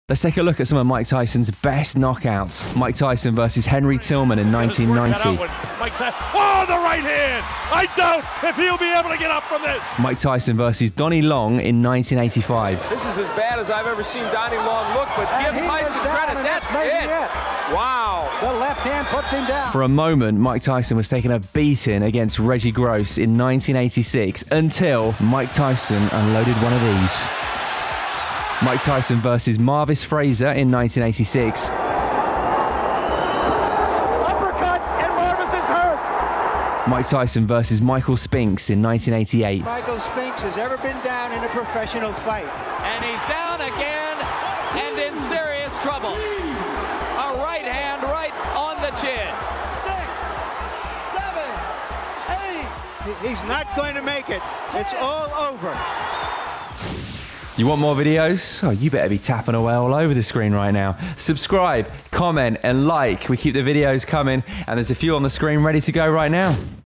Mike Tyson TOP 5 KNOCKOUTS
Mike Tyson Best Knockouts!

Mike Tyson vs. Henry Tillman (1990)
Mike Tyson vs. Donnie Long (1985)
Mike Tyson vs. Reggie Gross (1986)
Mike Tyson vs. Marvis Frazier (1986)
Mike Tyson vs. Michael Spinks (1988)

What’s your thoughts on Mike Tyson’s boxing style!? Was Mike Tyson natty or not!?